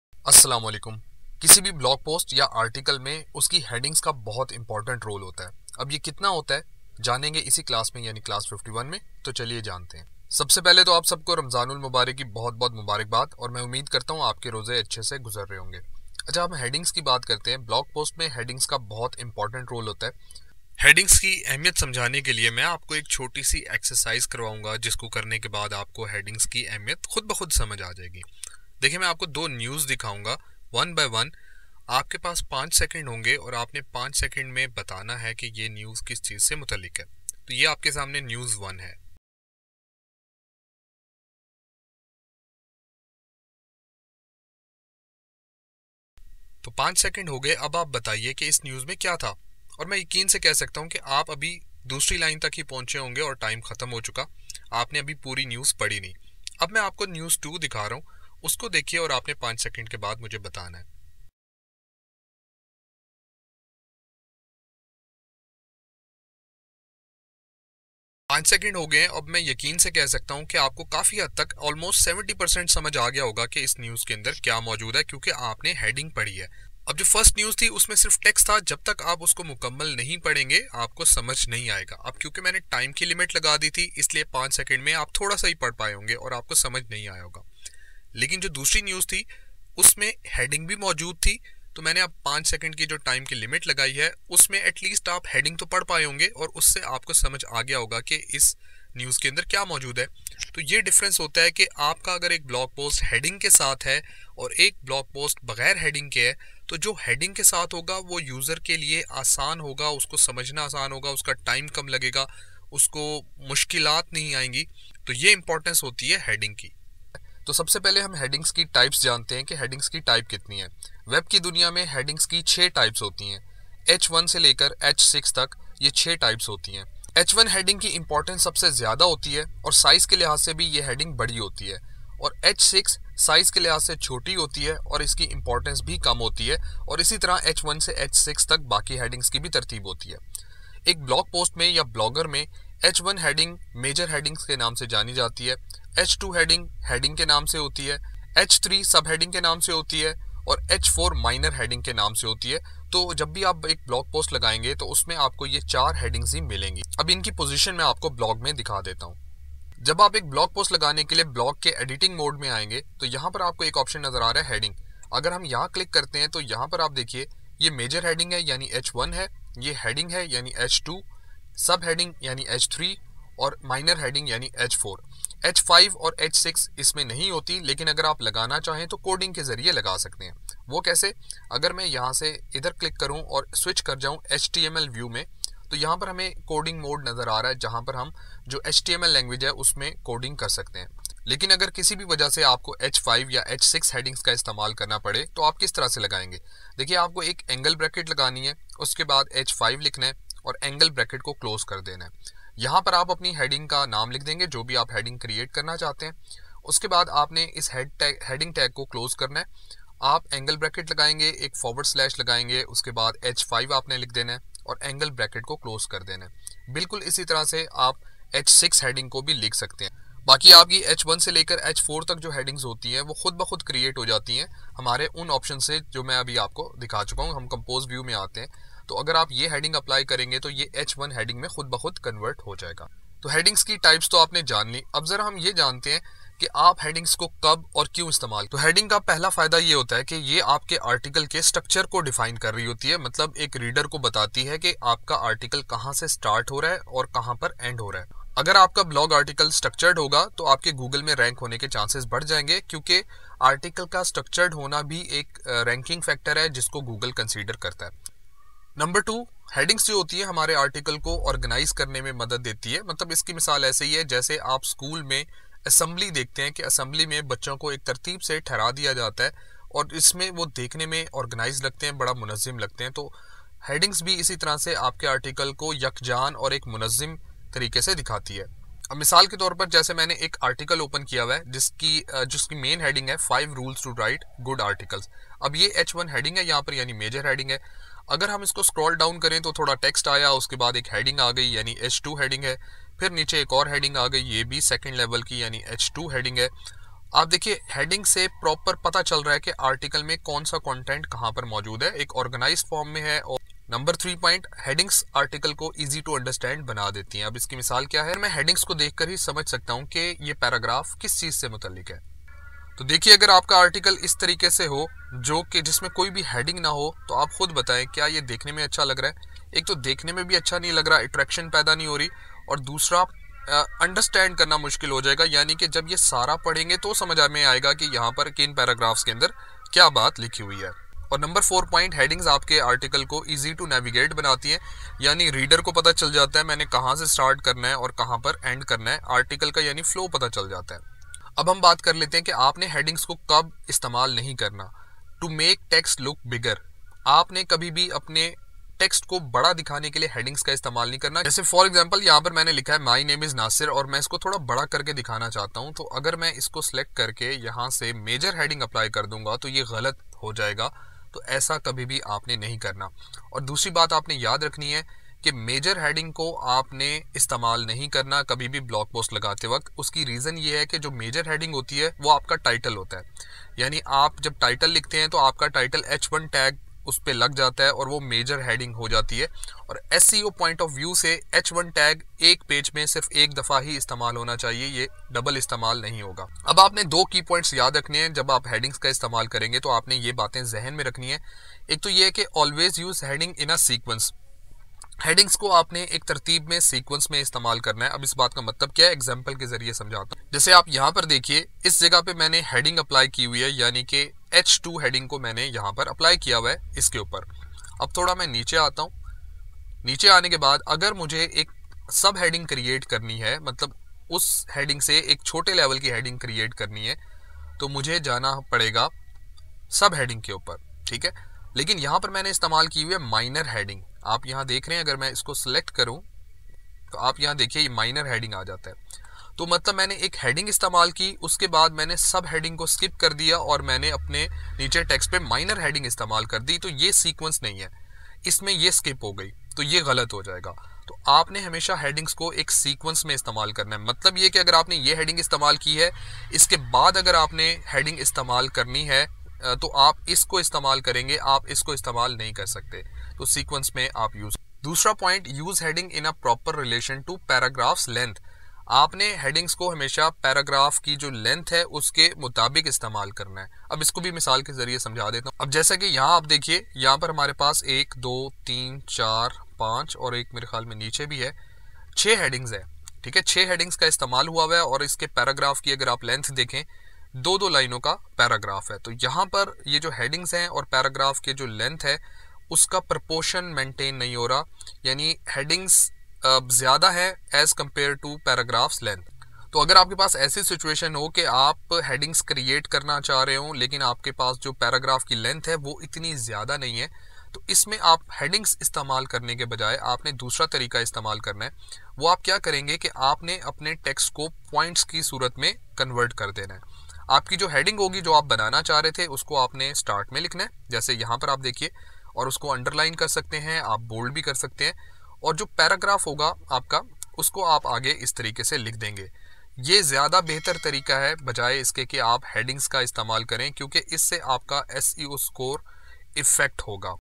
Importance and Types of Headings | On Page SEO | Blogging Course 2023
You will learn about the importance of heading in a blog article. There are 6 types of heading in the world of web. H1 to H6. Understand when to use heading and when not to use headings.

𝗡𝗘𝗫𝗧 𝗖𝗟𝗔𝗦𝗦 52:           COMING SOON!